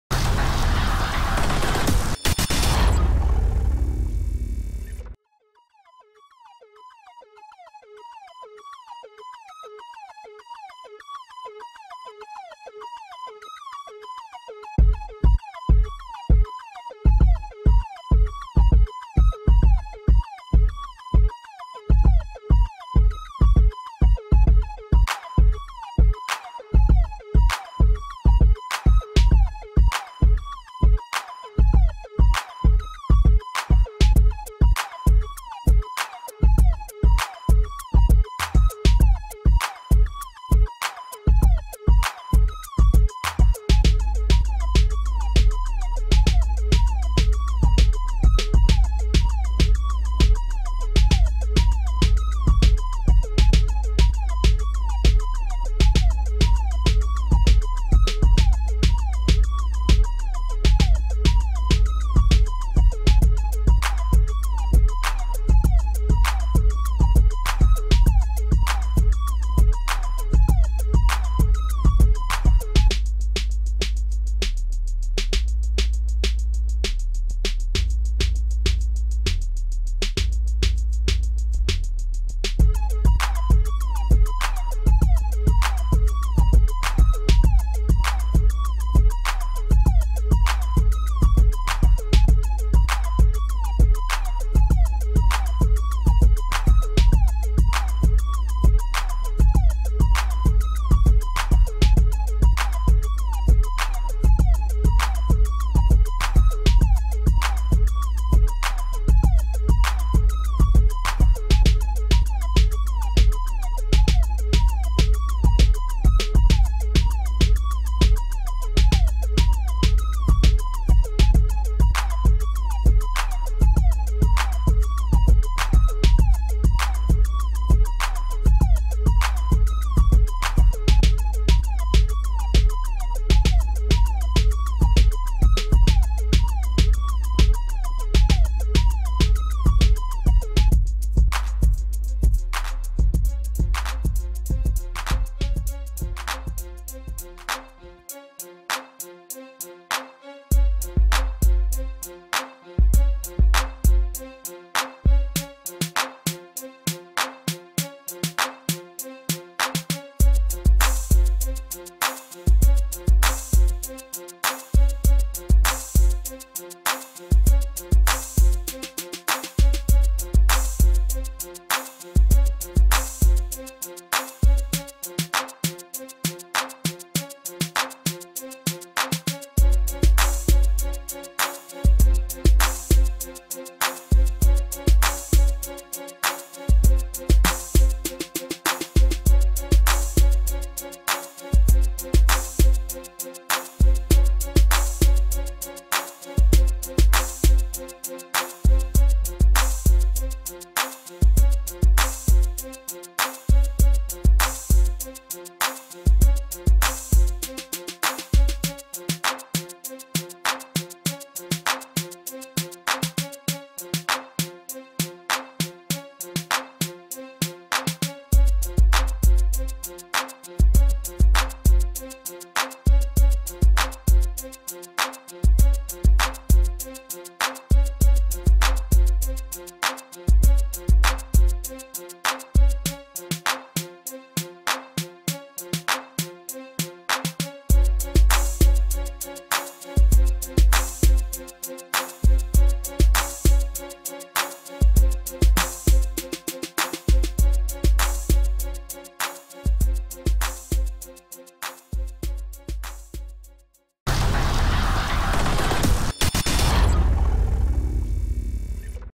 2015 Mississippi State Commits
Highlights of some of the top Mississippi State commits from the 2015 class.